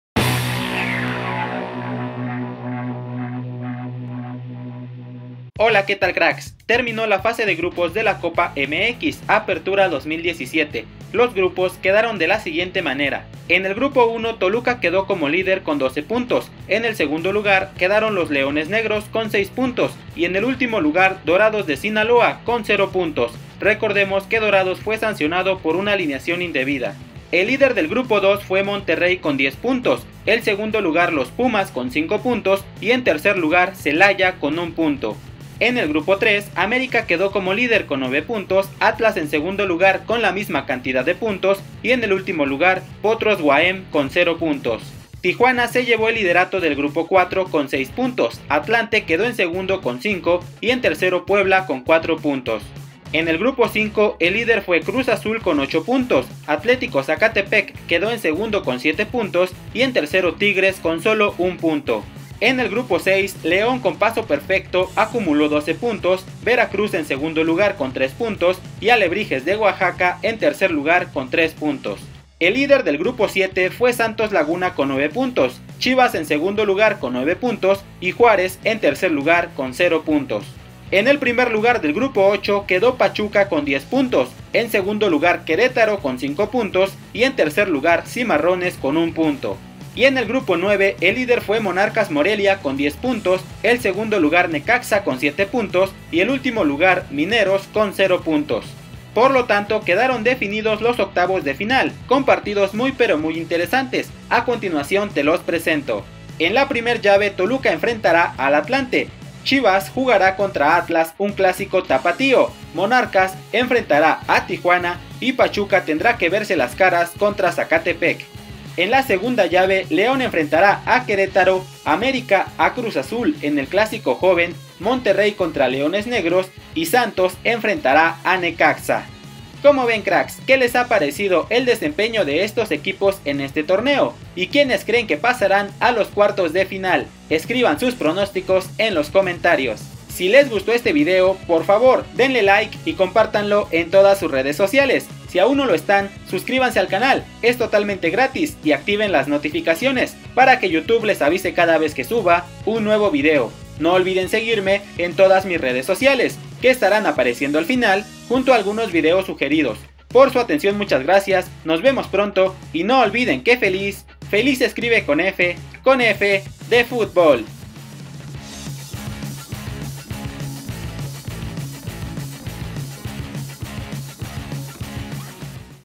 Así SE JUGARÁN los OCTAVOS DE FINAL en la COPA MX APERTURA 2017
Dos CLASICOS en los OCTAVOS DE FINAL de la COPA CORONA MX APERTURA 2017, el CLASICO TAPATÍO y el CLASICO JOVEN son sin duda los PARTIDOS mas IMPORTANTES en la FASE FINAL de la COPA MX.

Octavos de Final:

TOLUCA VS ATLANTE
ATLAS VS CHIVAS
MONARCAS VS TIJUANA
PACHUCA VS ZACATEPEC

LEÓN VS QUERETARO
AMÉRICA VS CRUZ AZUL
MONTERREY VS LEONES NEGROS
SANTOS VS NECAXA